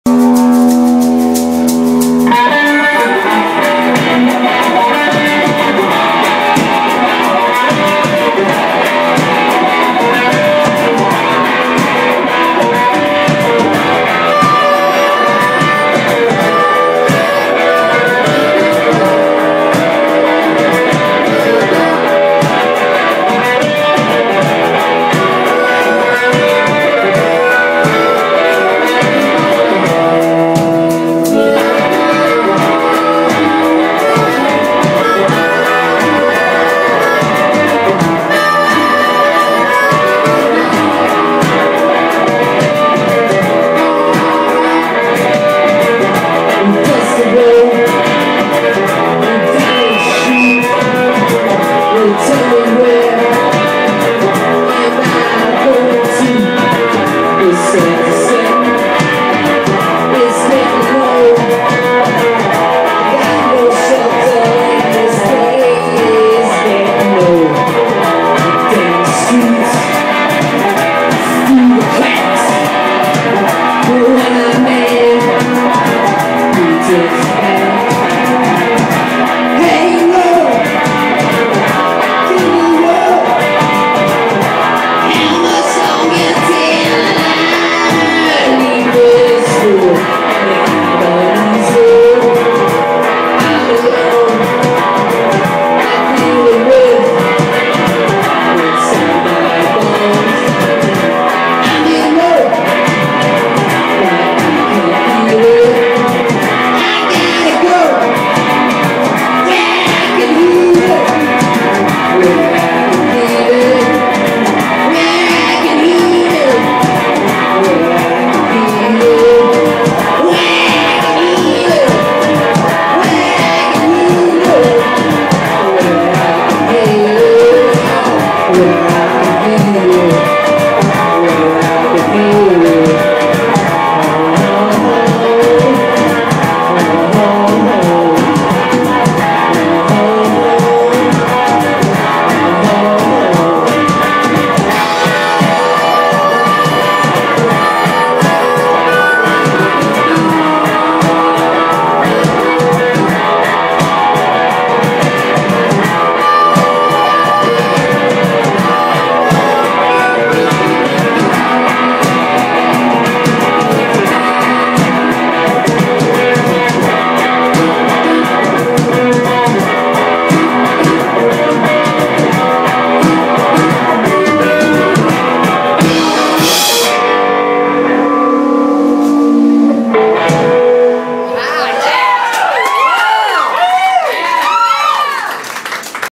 Lantern, Live @ Kung Fu Necktie Philadelphia 071112
Lantern plays live at KFN in Philly